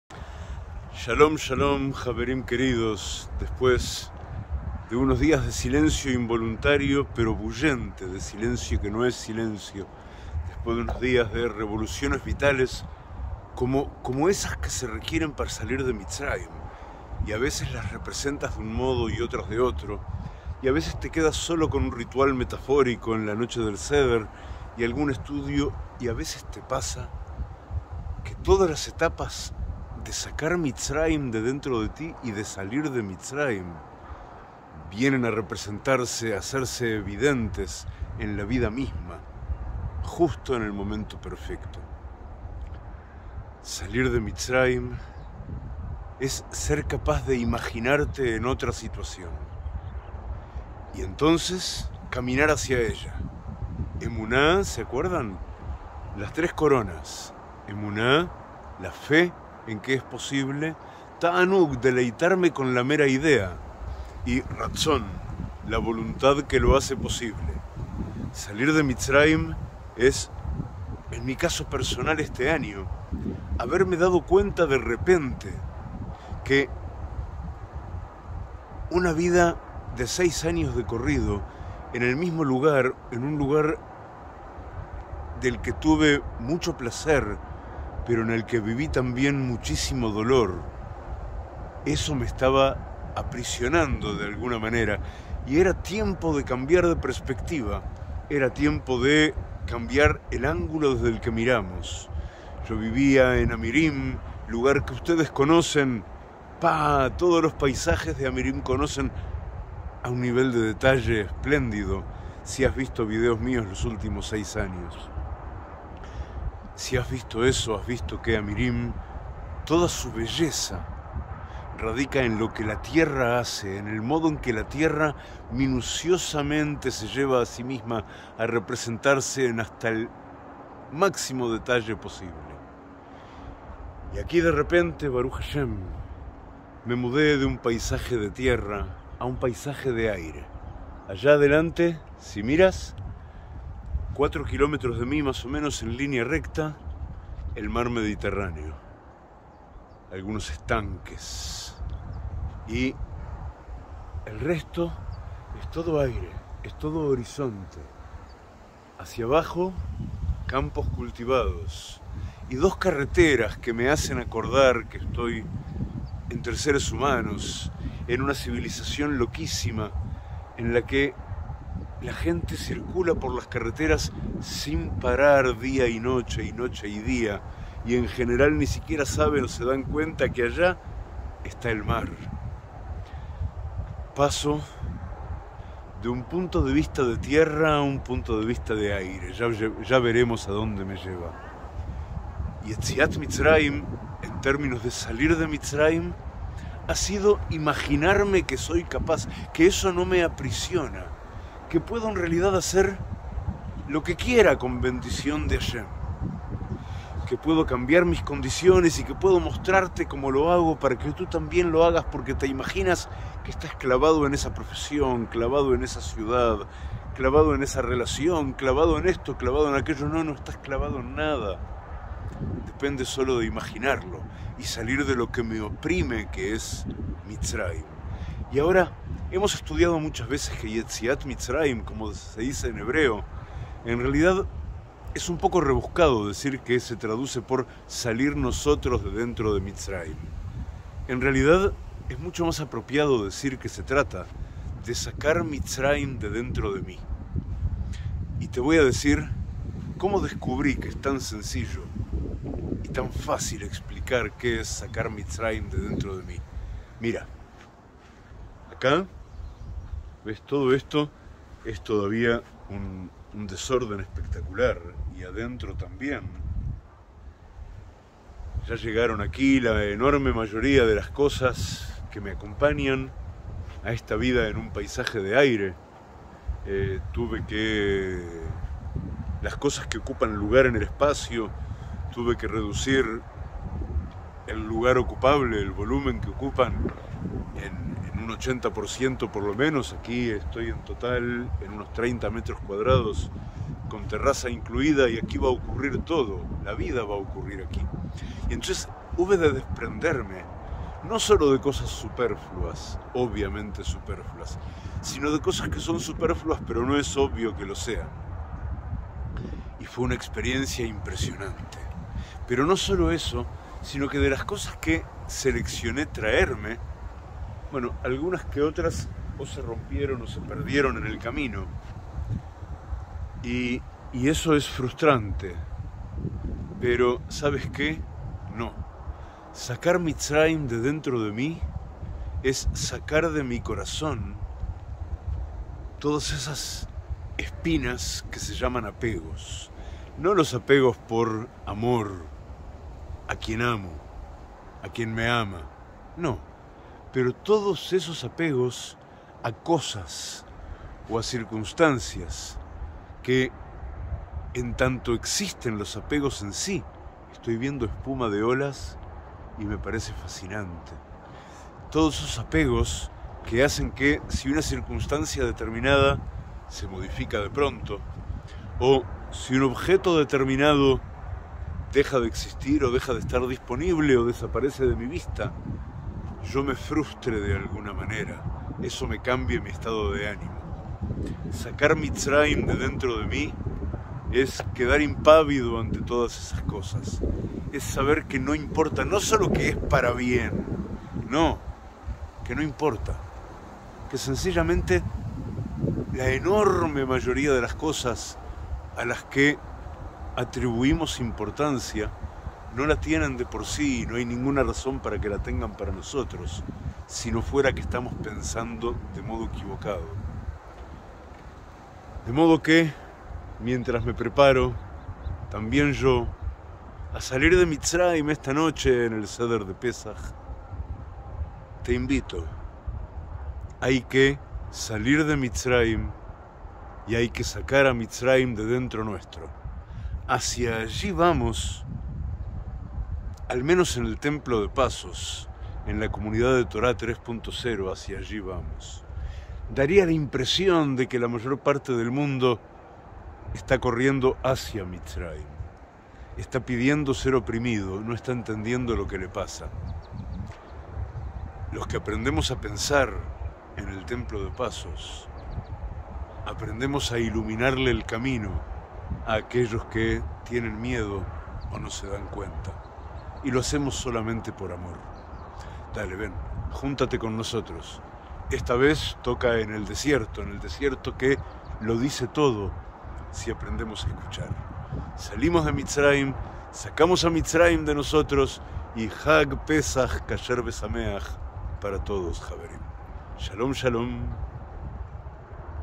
¡Pésaj! Mitsráim no tiene lugar en mí
Pesaj 5782, en el BarcoPirata de ElTemplodePasos
Te espero en la Tertulia en el Patio del Templo de Pasos